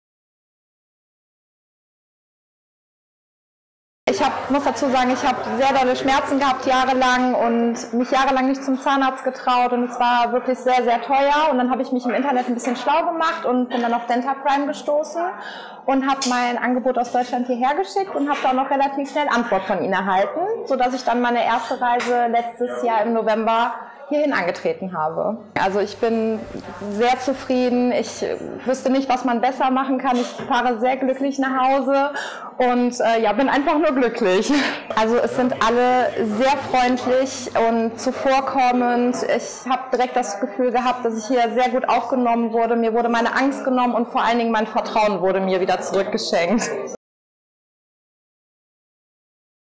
Dentaprime Zahnklinik Erfahrungen: Nadine Lammers | Neue Zähne im Ausland
Behandlung: Brücken und Kronen aus Zirkonoxid